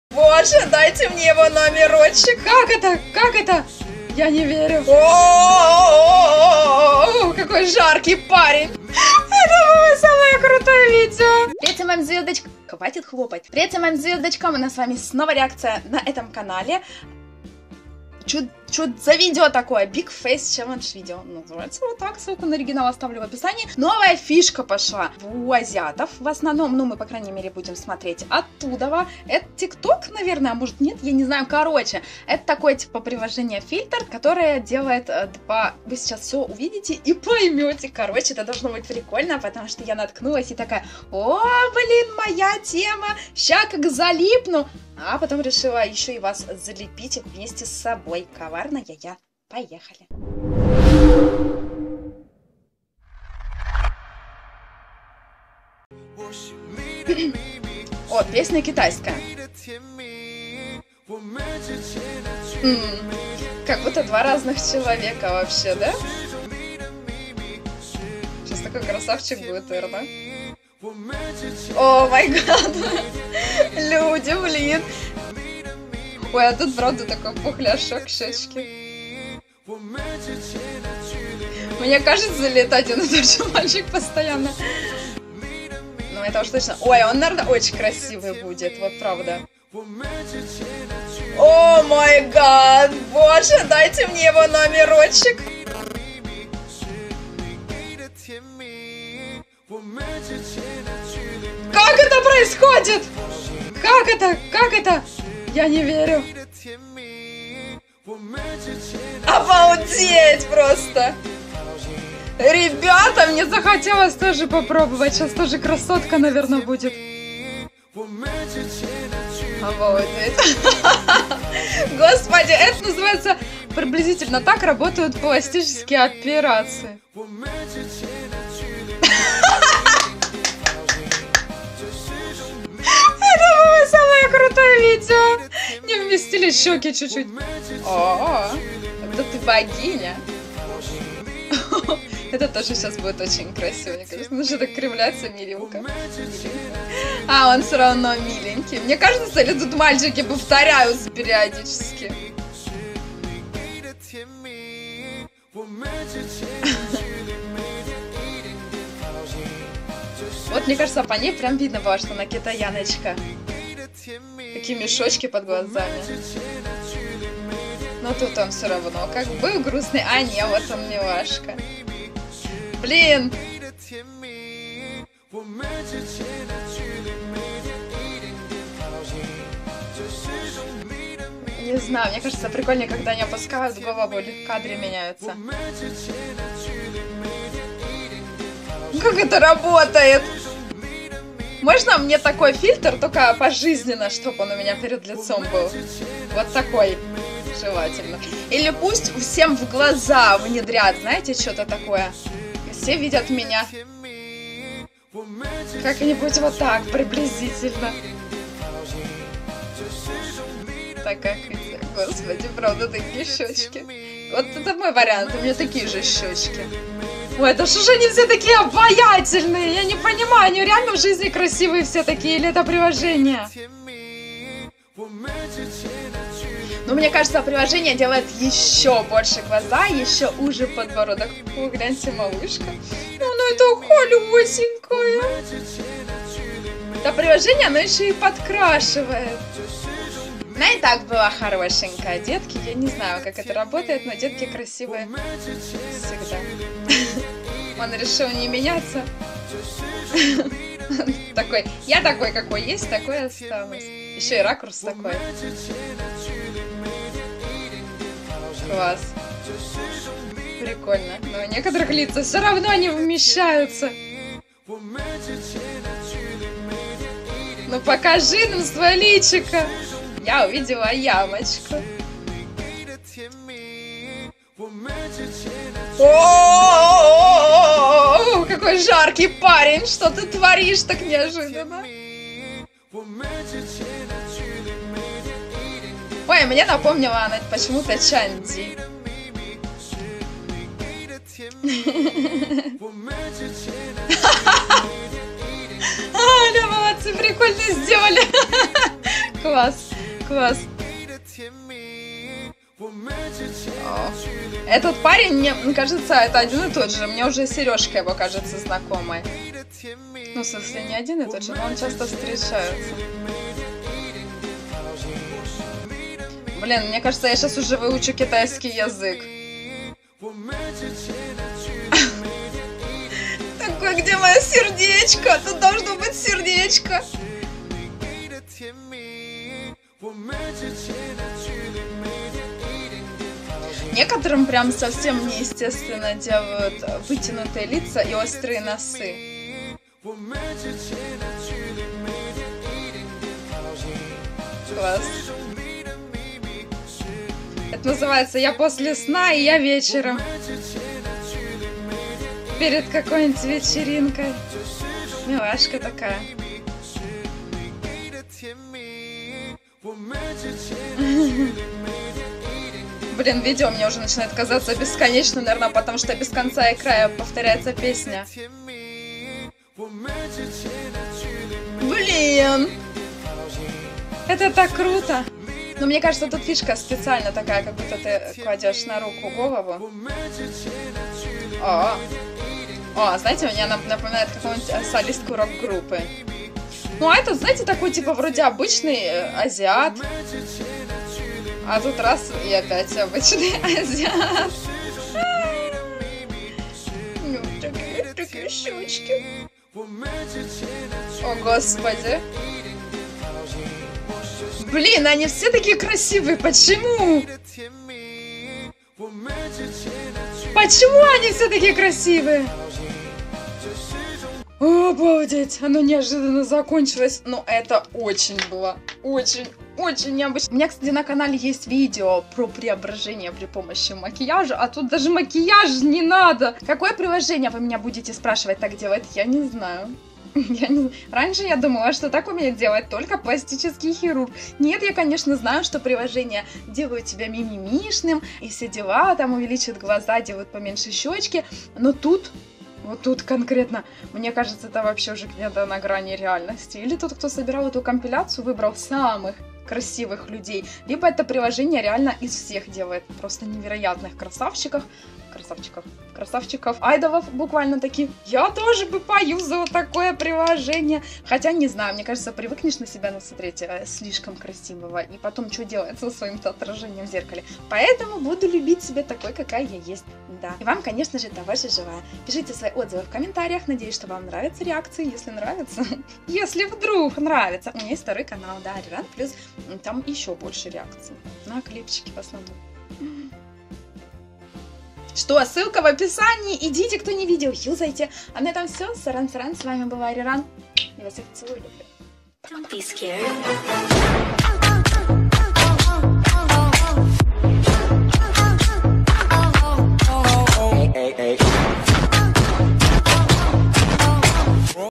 БОЛЬШОЕ ЛИЦО ЧЕЛЛЕНДЖ | ARI RANG
Привет! В последнее время среди азиатов всё больше пользуются популярностью приложения, меняющие внешность до неузнаваемости. Давайте вместе посмотрим компиляцию Big Face Challenge До и После :)